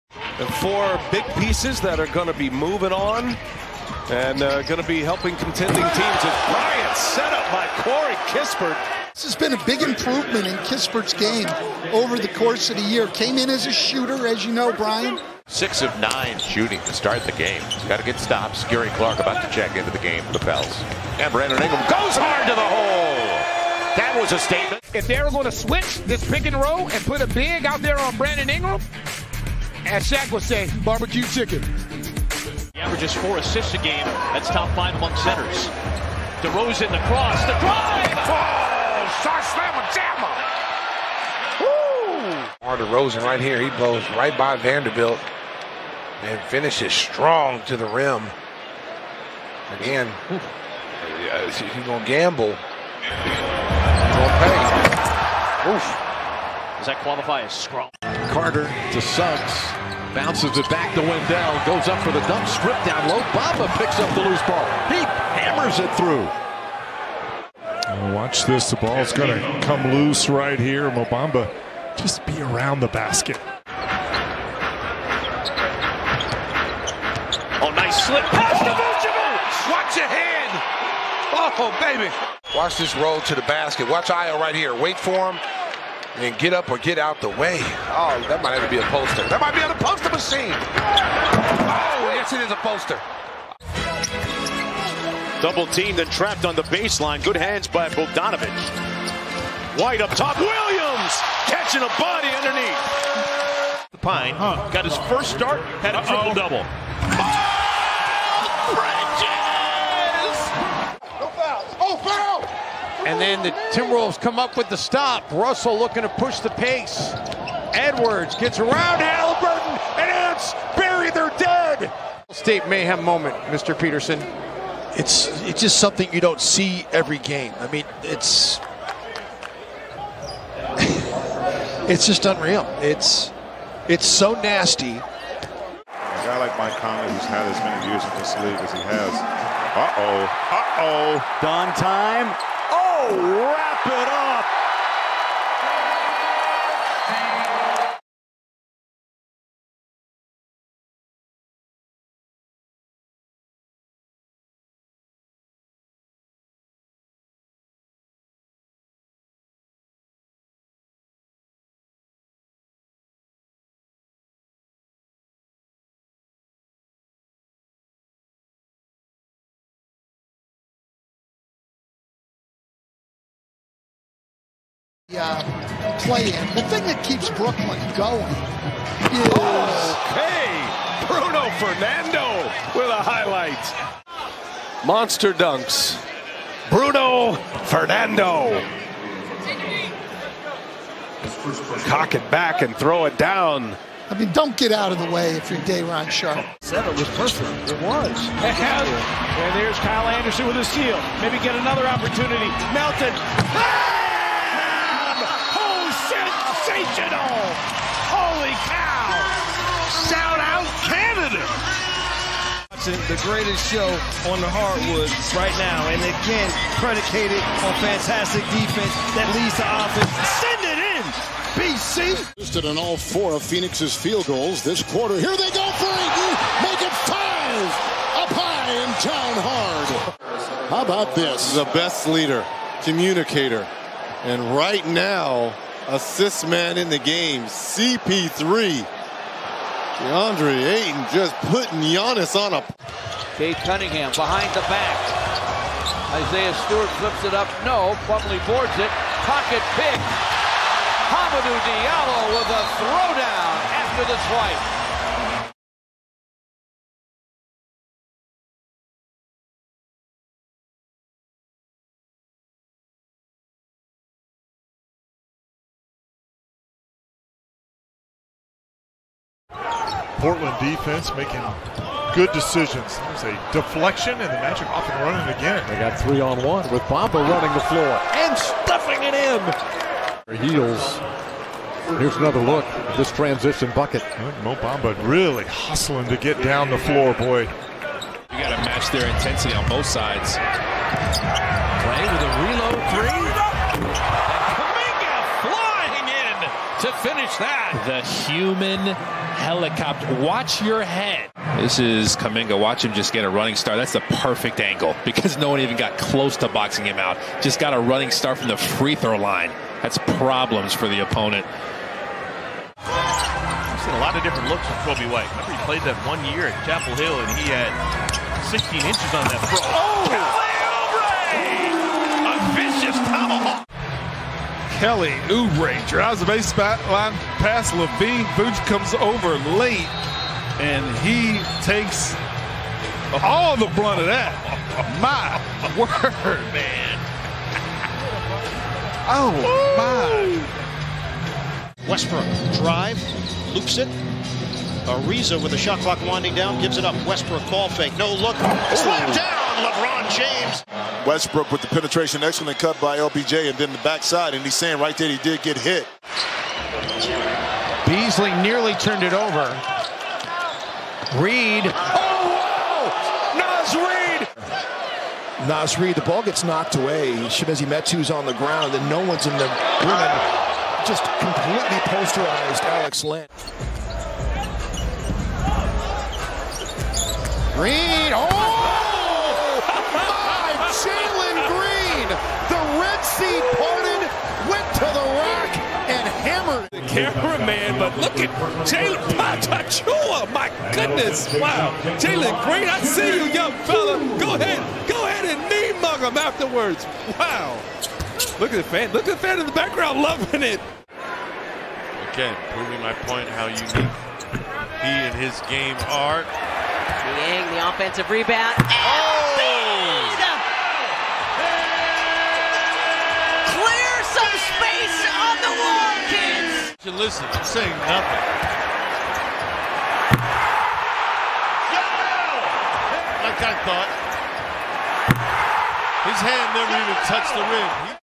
Best dunks of Week 17
Check out the top dunks from Week 17 of the NBA season!
PLAYERS: Kelly Oubre Jr.  Brandon Ingram  Donovan Mitchell  Thomas Bryant  Mohamed Bamba  Hamidou Diallo  Bruno Fernando  Deandre Ayton  Robert Williams III  Brandon Clarke  Nazreon Reid  Anthony Edwards  Jalen Green  Jonathan Kuminga  DeMar DeRozan  Nikola Vucevic  Joel Embiid  LeBron James